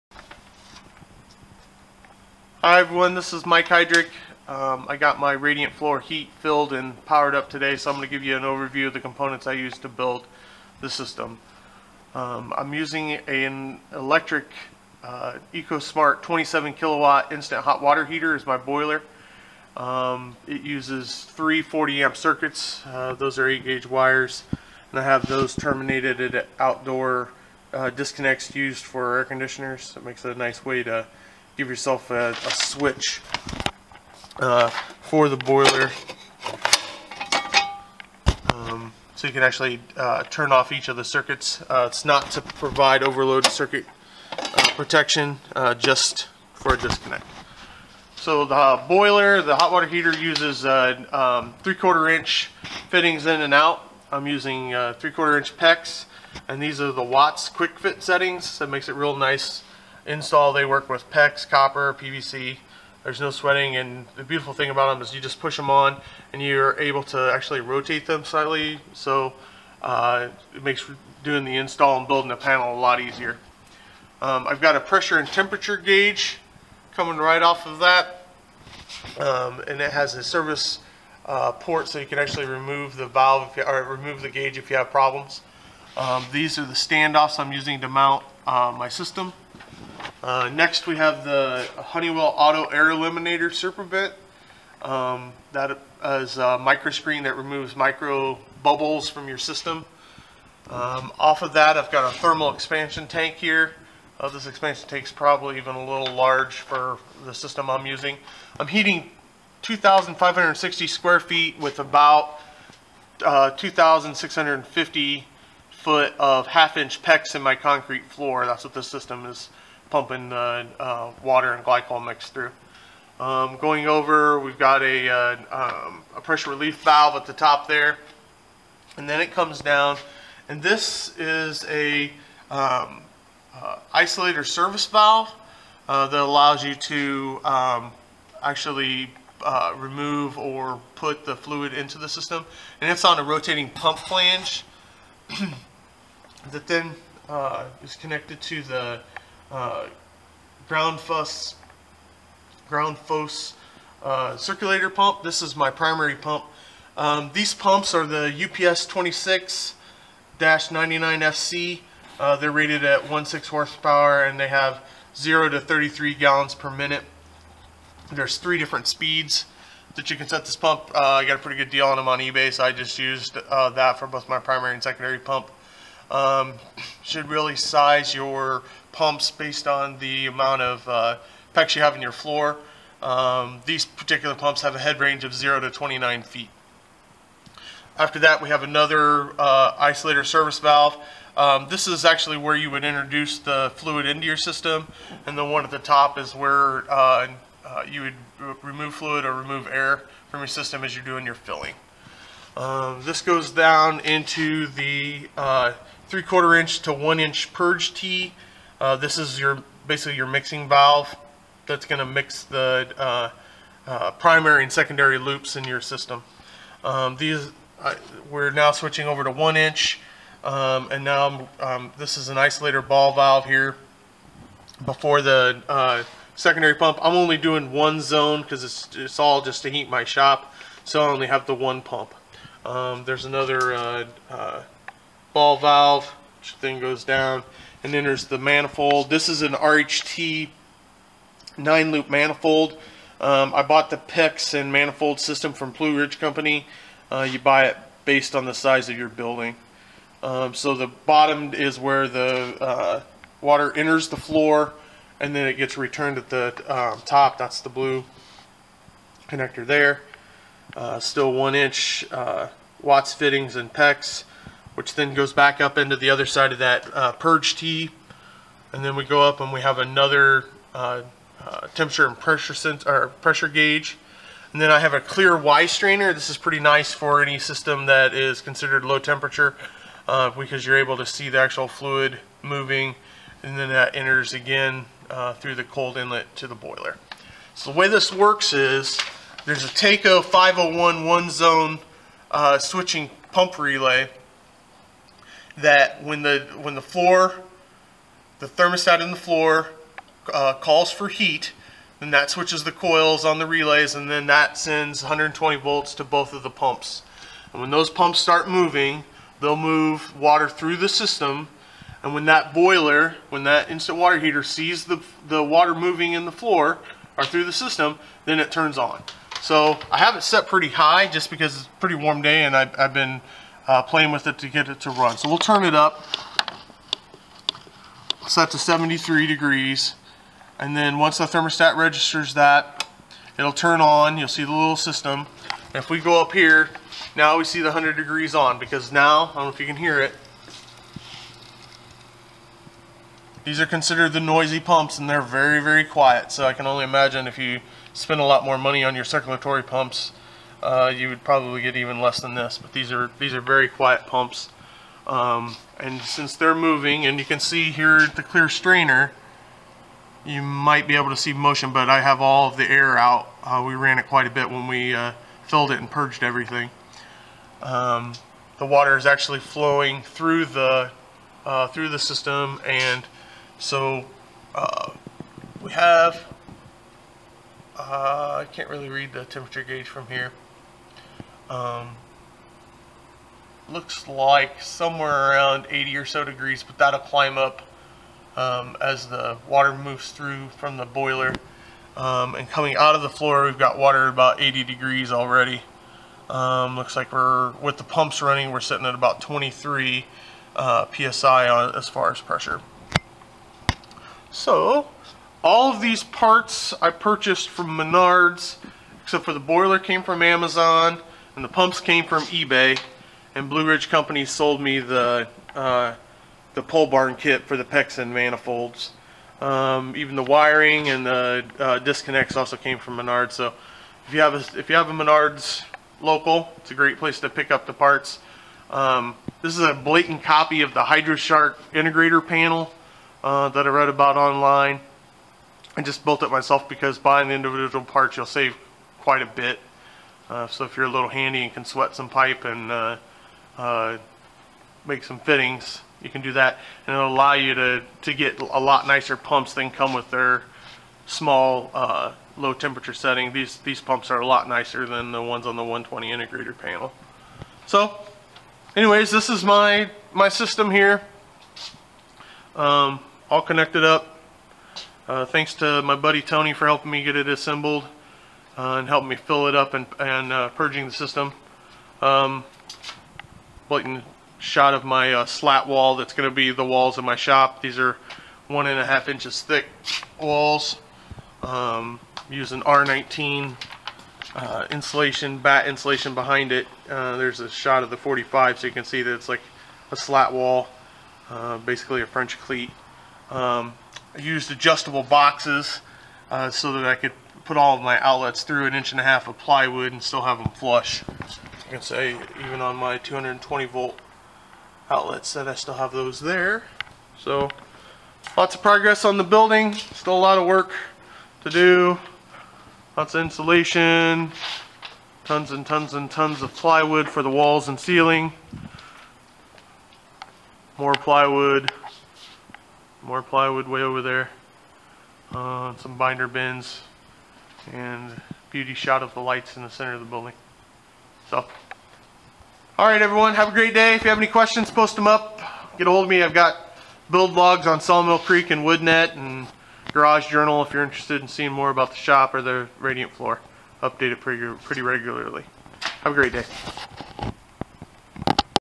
Radiant Heat Components Overview
A walk through of the radiant floor heat components I used to build my hydronic heat panel and will use to heat my 2560 sqft concrete floor/shop.